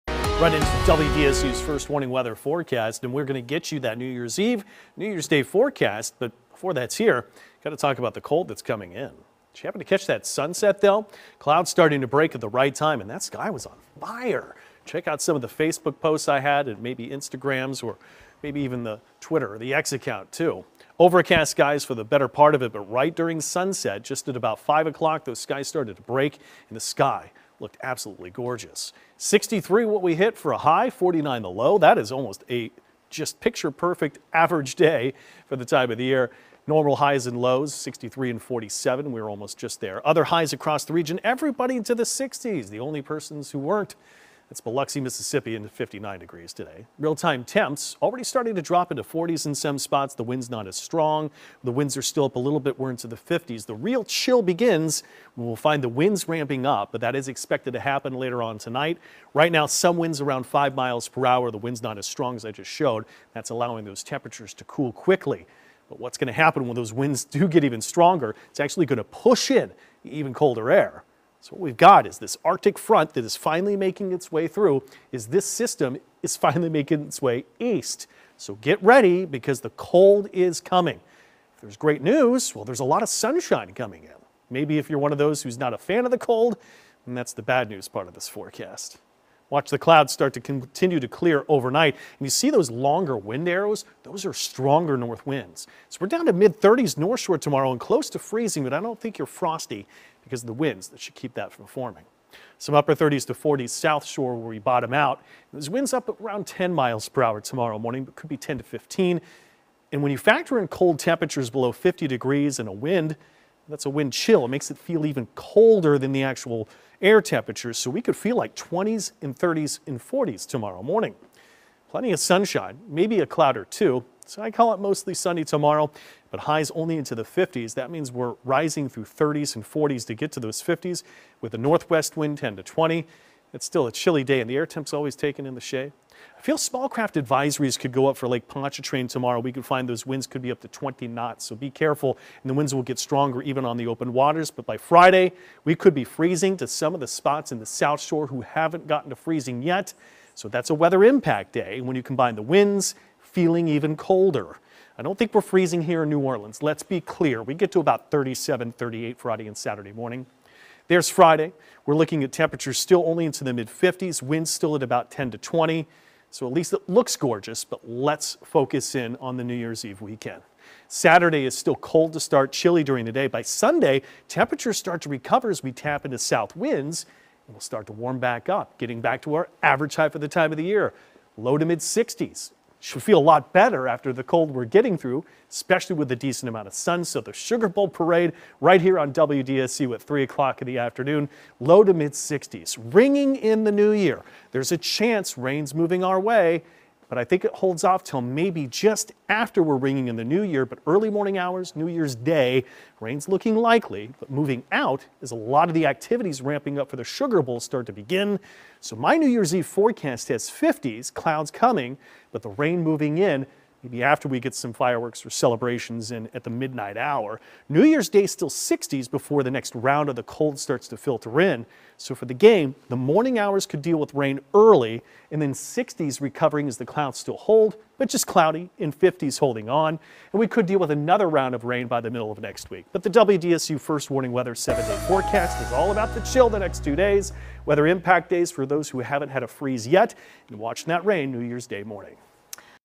Colder days ahead, possible freezes, and possible rain around the New Year
Meteorologist Devon Lucie dives into the colder air that spills into the region over the next several days identifying who's likely to find freezing days, when they're here, and how long the cold will last, while tracking our next round of rain that could impact New Year's Eve celebrations and New Year's Day events.